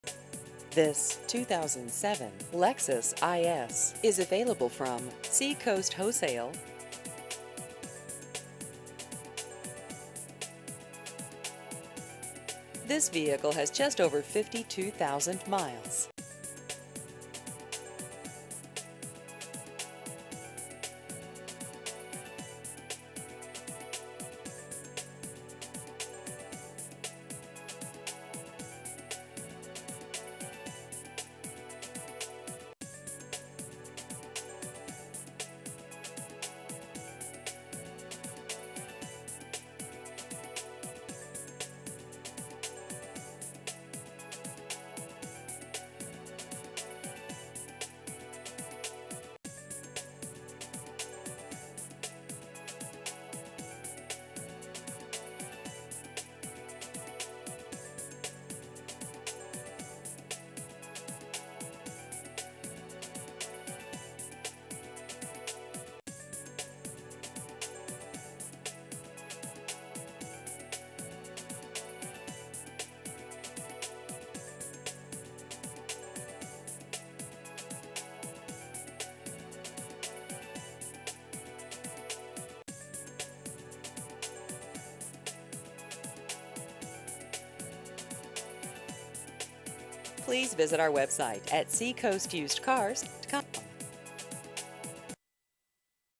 2007 Lexus IS Used Cars Hampton Falls NH
This 2007 Lexus IS is available from Seacoast Wholesale.